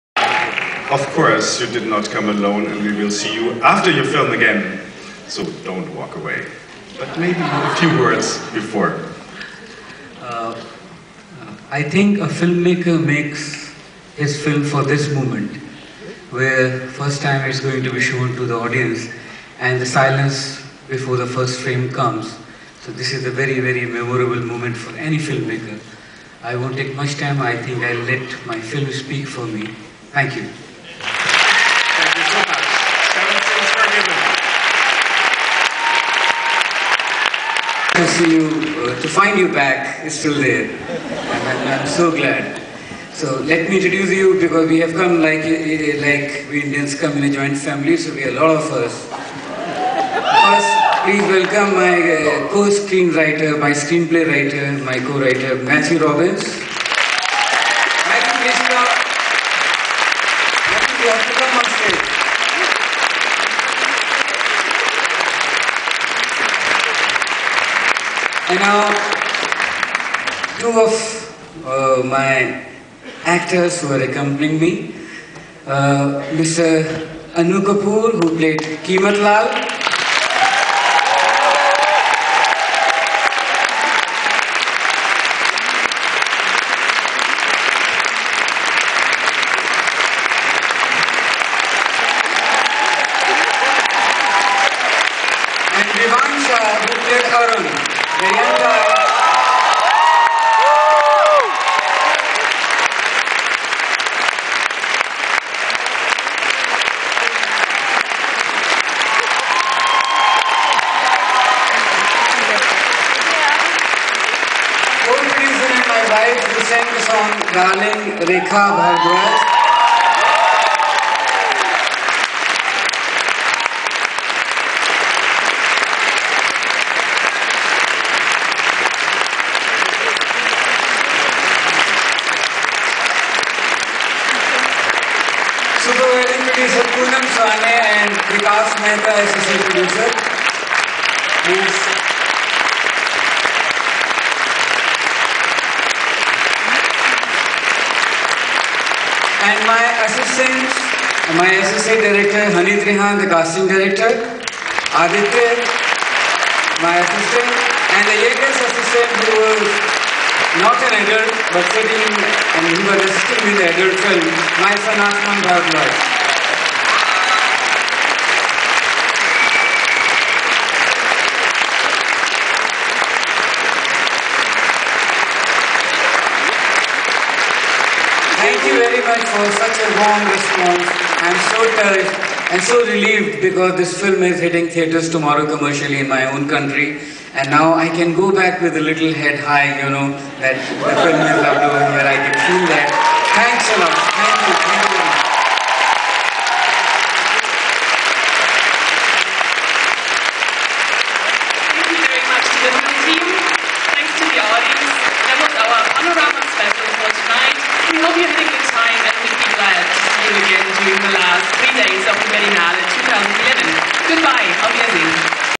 Berlinale 2011: 7 Khoon Maaf
Regisseur und Produzent Vishal Bhardwaj spricht bei der Weltpremiere von 7 Khoon Maaf zum Berlinale Publikum, auch einige Darsteller sind anwesend